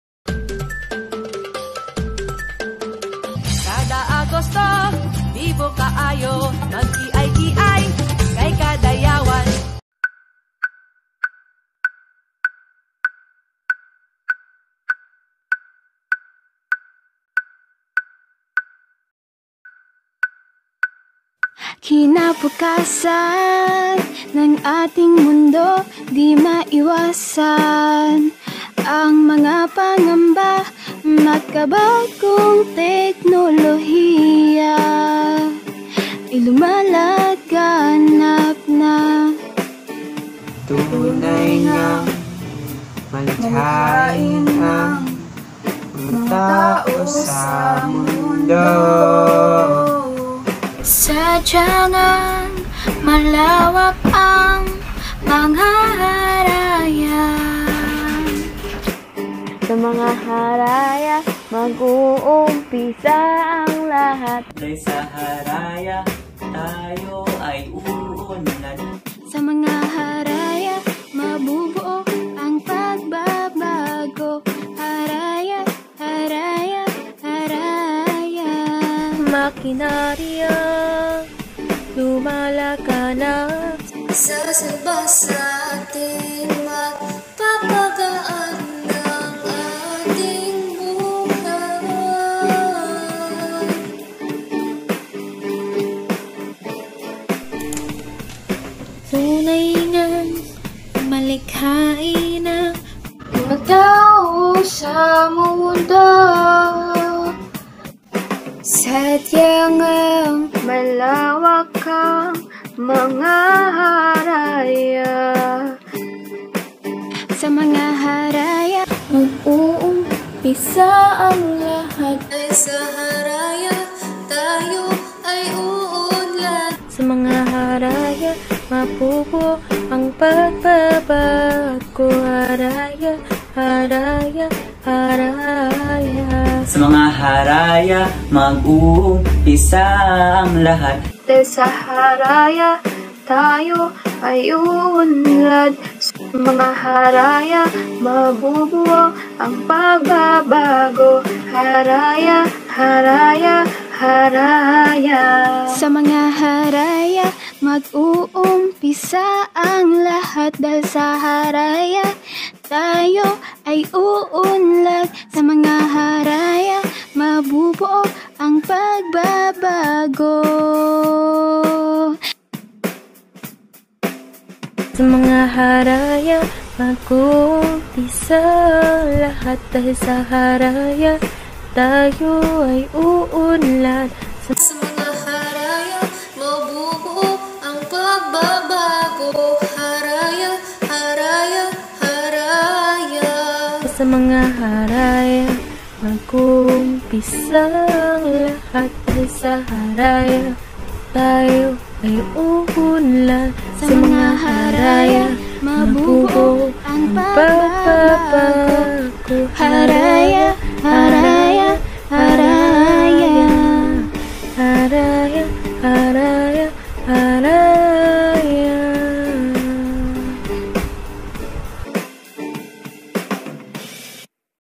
G11-Kadayawan Jingle Making Entry
Science Month Celebration
Science and Technology: Championing Health, Livelihood, Harmony for the Future
Science Jingle Making Contest Grade 11 - Category